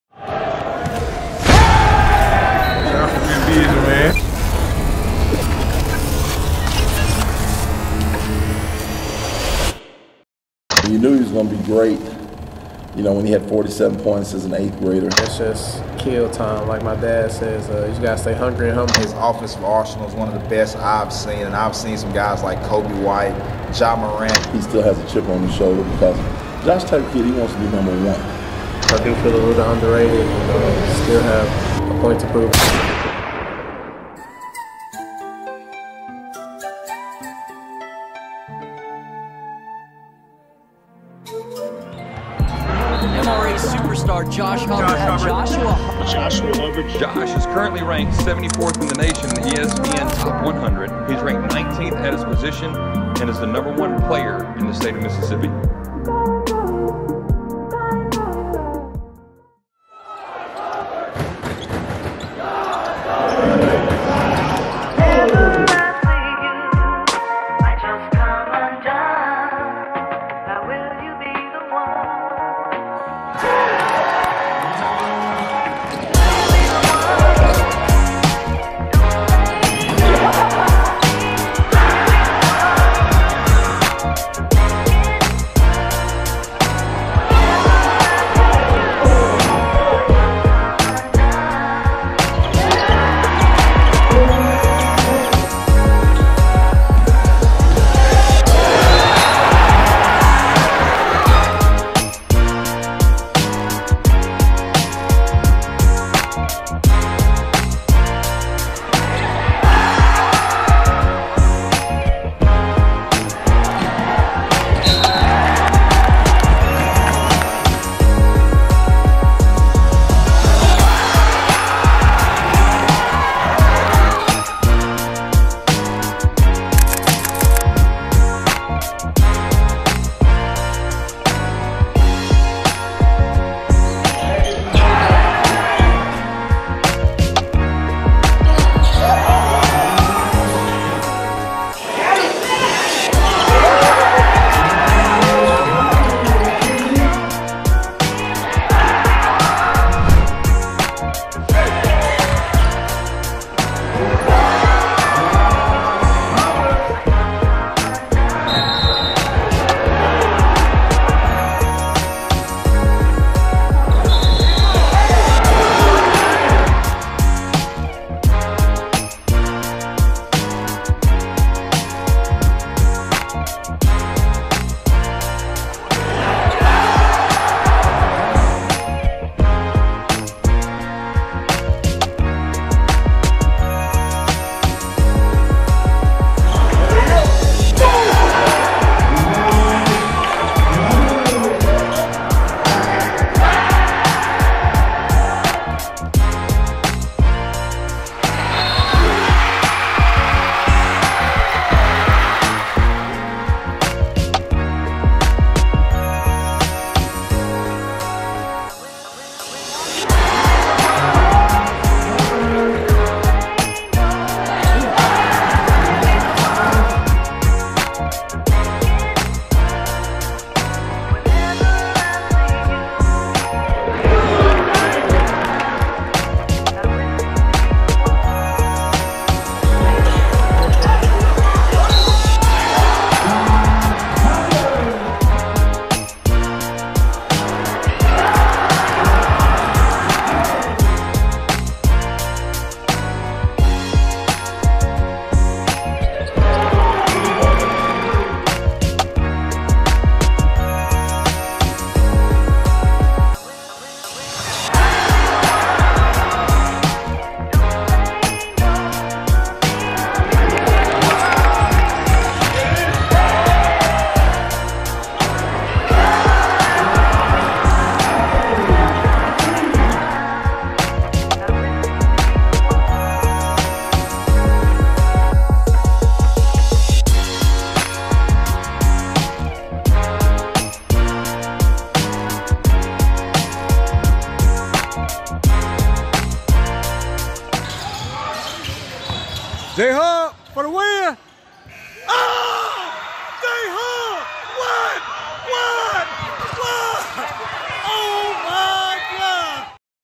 Mississippi ALL-TIME Leading Scorer & Mississippi State Commit Josh Hubbard High School Mixtape!!
Josh's storied high school career will be remembered forever in Mississippi. Josh not only holds many division-one offers, but he has also  won Gatorade Player of the Year, is a multi-time Dandy Dozen selection, Maxpreps Boys Basketball Player of the Year, Mississippi's all-time leading scorer, and has won several state championships. Congrats, Josh and best of luck at the next level!